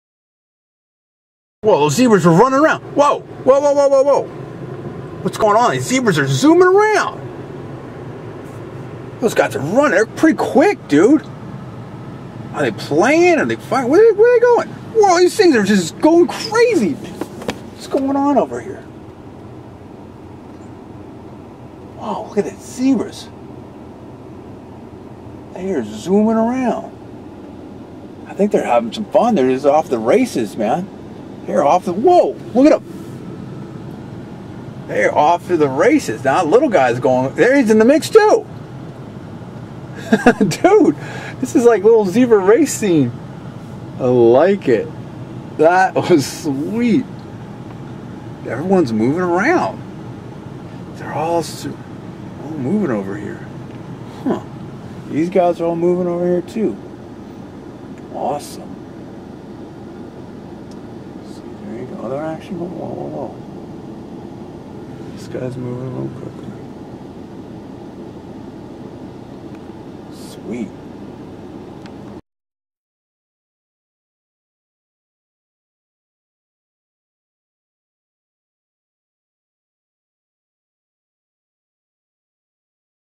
Zebras Chasing Other Zebras - Plus One Zebra Kicks at Another Zebra - in 1080P HD
Also, at the beginning one zebra kicks at another zebra.

John D. Villarreal saw these incredible animals at Lion Country Safari in Florida.